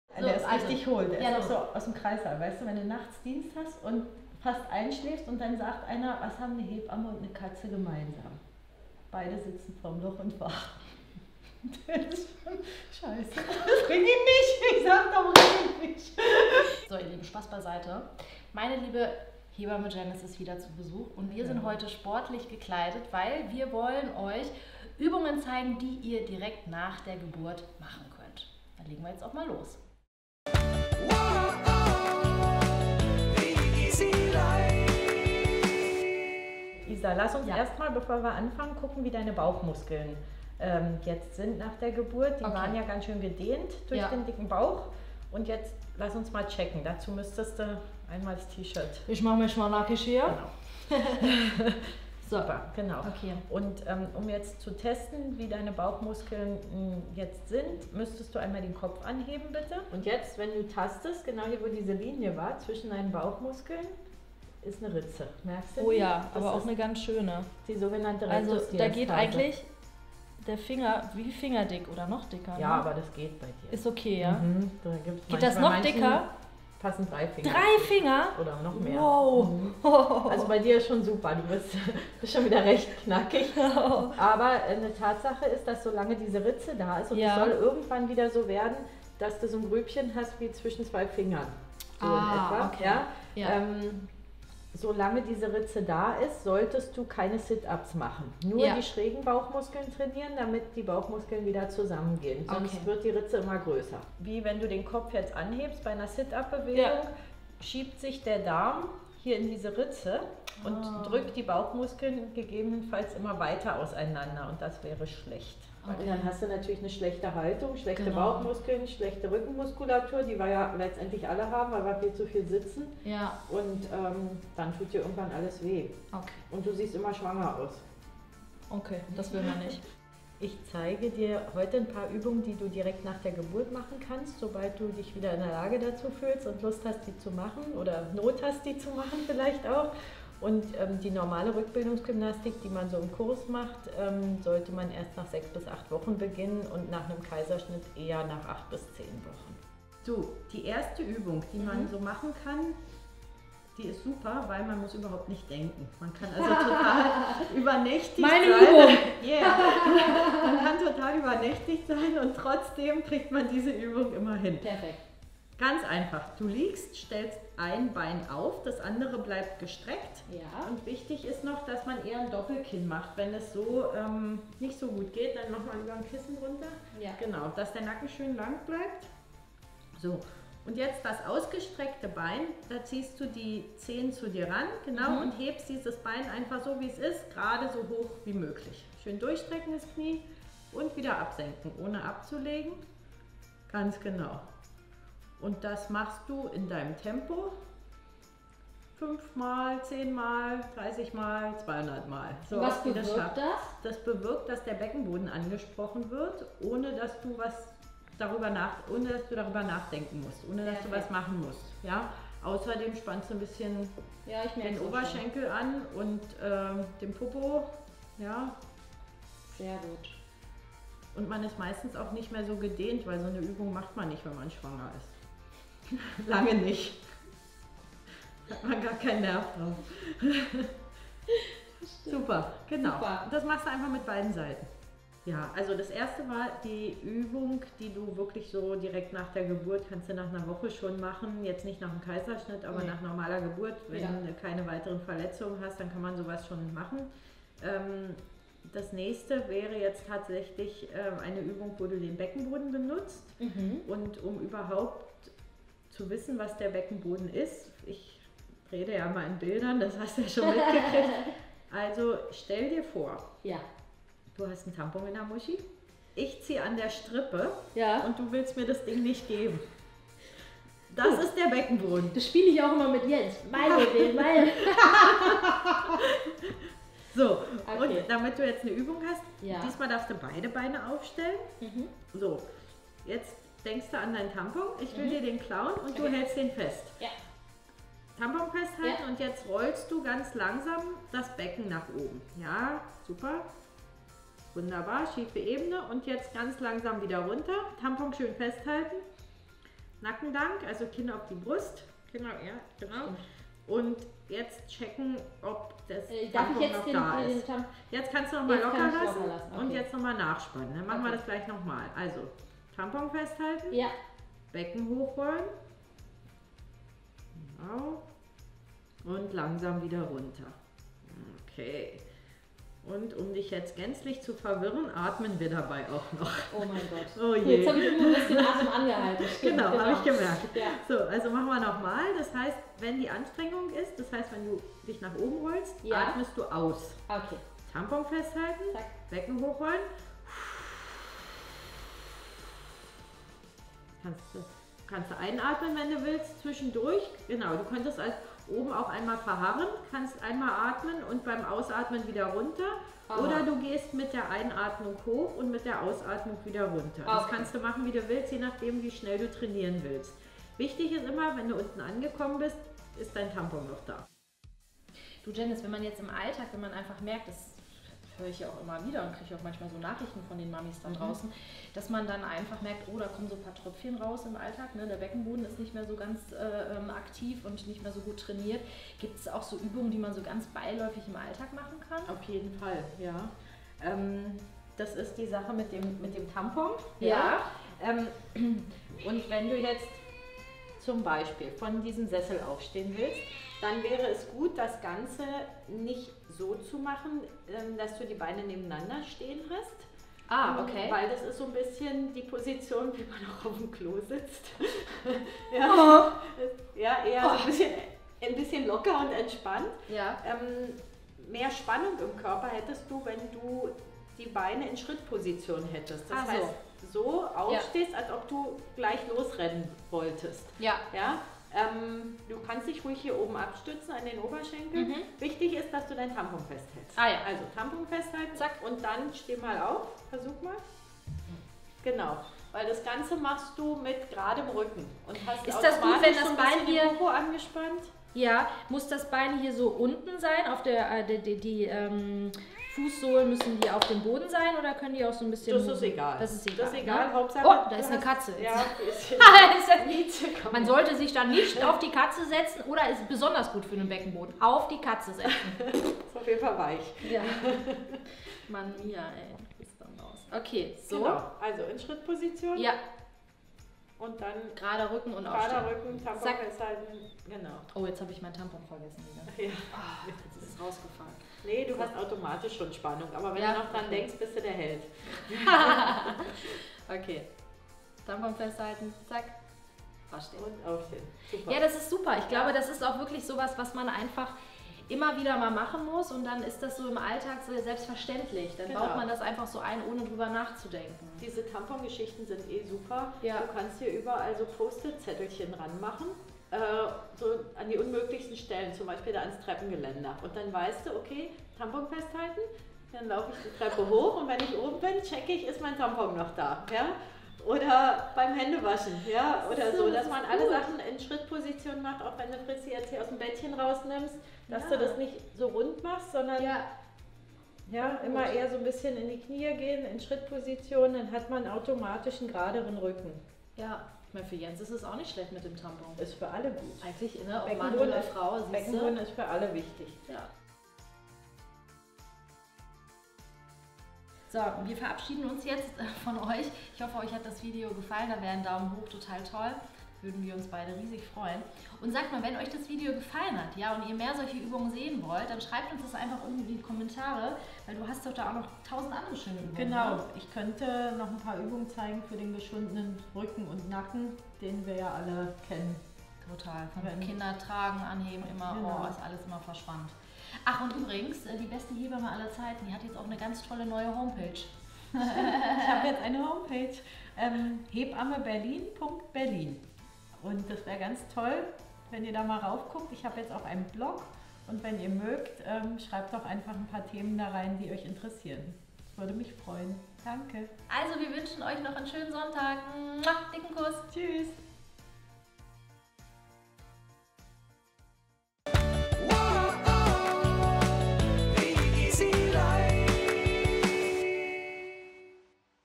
Erstes Beckenboden Workout nach der Geburt
heute wird es mal wieder sportlich bei "The Isi Life". Meine liebe Hebamme Janis und ich zeigen euch tolle, effektive Beckenboden Übungen, die ihr nach der Geburt prima in euren Alltag mit Baby einbauen könnt.

Wir freuen uns über euer Feedback und wünschen euch einen wundervollen Sonntag,
Isi ❤️

Wenn ihr Lust habt, schaut gerne mal auf der neuen Homepage von Janis vorbei